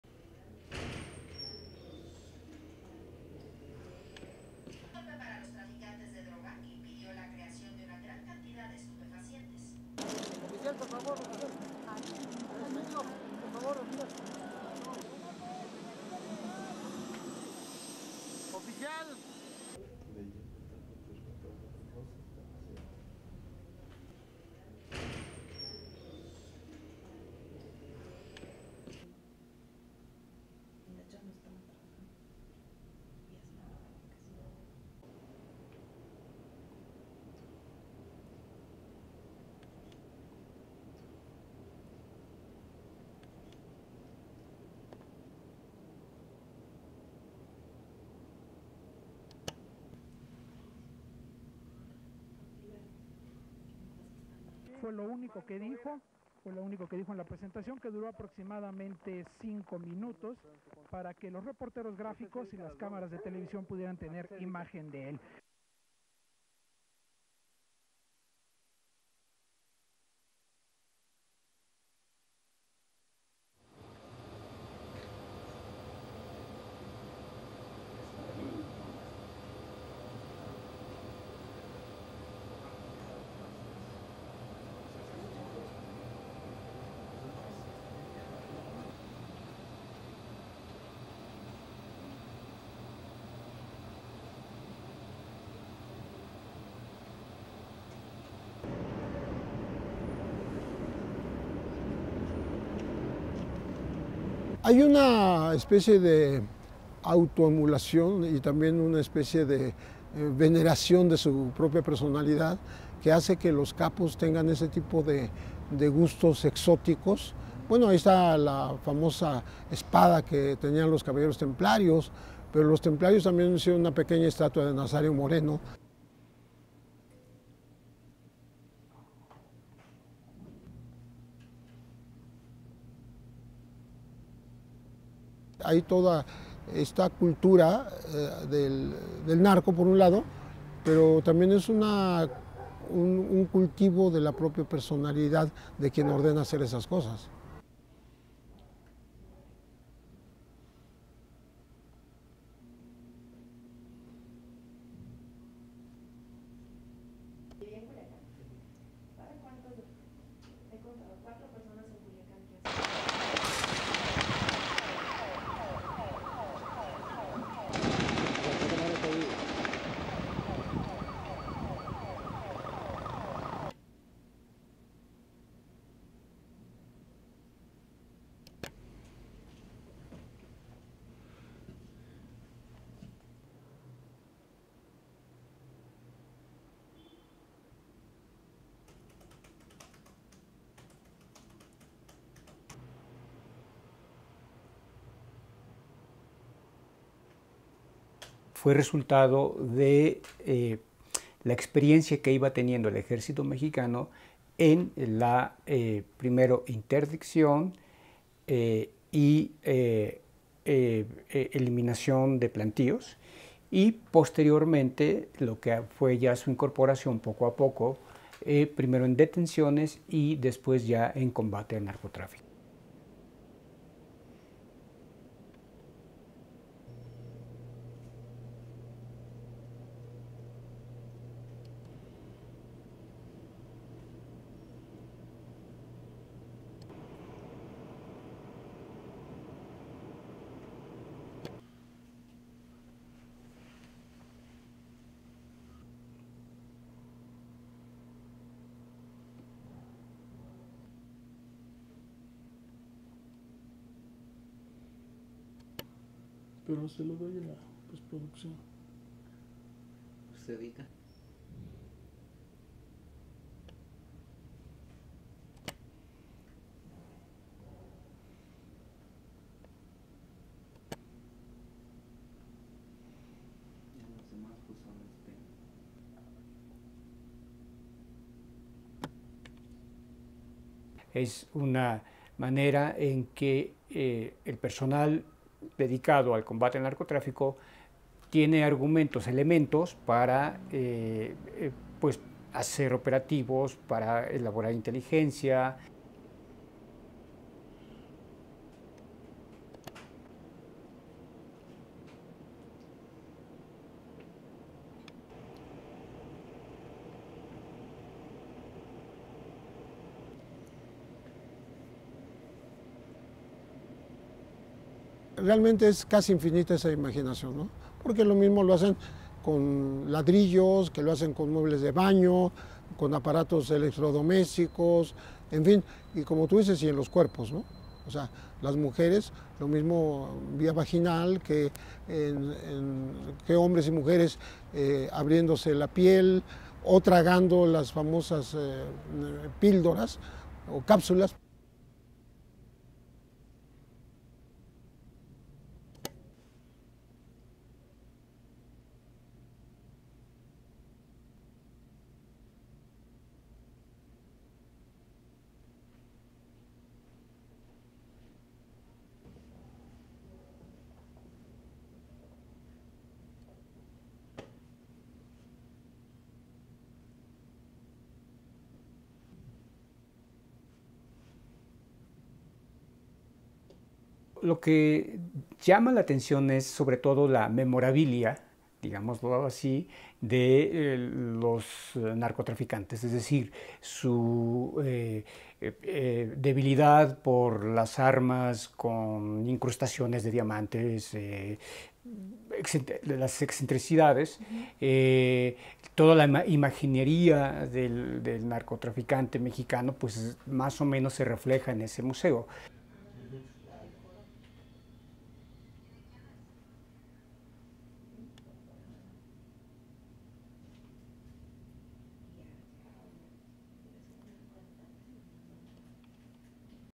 A rare look inside Mexico's secret drug museum
(2 Feb 2020) LEAD IN:
Mexico's long-standing war against drug cartels has inspired a bizarre collection of items in a museum that is not open to the public.
The secretive exhibition can only be accessed by military and intelligence agents and occasional journalists. Its purpose: to better understand drug lords and stop their illegal activities.

STORY-LINE:
This a diamond embedded Colt 45 with Joaquin "El Chapo" Guzman's initials that was confiscated during his 2016 arrest.
It is part of an unusual collection at the Museum of Drugs, inside of the National Defense Ministry office in Mexico City.
Other illicit treasures on show include a gun which once belonged to the drug lord Amado Carrillo, also known as the "Lord of the Skies", allegedly a gift he gave to El Chapo Guzman and was seized when he was captured in Guatemala.
Guzmán was convicted on drug conspiracy charges in New York. He was sentenced last year (2019) to life in prison.
Amado Carrillo died in 1997 in a plastic surgery operation to change his appearance.
But this museum is not open to the public. It is for the exclusive use of the Mexican army and intelligence units, who may marvel at the stuffed lion and glittering jewels, but also come to learn about the drugs and the weapons that the drug traffickers use.
Journalist Jose Reveles is a journalist who has written books about Mexico's drug traffickers.
He says their large egos inspire their taste for opulence and self glorification.
"There is sort of a self-emulation and also a sort of veneration of their own personality that makes the drug lords have these exotic tastes," he says.
"There is the famous sword that the Templar Knights (a cartel) had but also the Templar had made a little statue of Nazario Moreno."
On March 9, 2014 Mexican soldiers killed Nazario Moreno Gonzalez, leader of the Familia Michoacana, and later the pseudo-religious Knights Templar cartel, while riding a mule in the remote Michoacan mountains.
The government had previously claimed that he was killed in December 2010, but never produced a body.
A Mexican government profile said Moreno "erected himself as the 'Messiah,' using the Bible to profess to poor people and obtain their loyalty."
The Drug Museum has a Knights Templar sword decorated with jewels next to a sculpture depicting Moreno as a saint.
"It is a cult of their own personality, of that person having all this stuff made," says Reveles.
Elsewhere, there is an altar dedicated to Jesus Malverde, the "patron saint" of drug traffickers.
The turn-of-the-century Mexican bandit, a legendary Robin Hood-like figure, has no saintly standing in the Roman Catholic Church. However, he enjoys a "good outlaw" image and near-cult status among the city of Culiacan's poor.
The exhibits shed light on the narco- lifestyle and their operations, as well as the development of narco-culture in Mexico, according to investigative journalist Jorge Carrasco.
"It (the museum) is the result of the experience the Mexican Army was gaining, first in the interception and elimination of drug crops and later, what was their inclusion little by little, first in the arrests and then in the war against drug-trafficking."
The Museum of Drugs is a National Defense Ministry collection which began in 1986. The exhibition spans 10 halls, covering the origin of drug trafficking up to the health problems caused by drugs, shown in their different forms, next to powerful weapons used by the cartels.
The museum is not a tribute to the drug lords.
Its primary function is to inform and educate officers on the front line of the war against drugs in Mexico.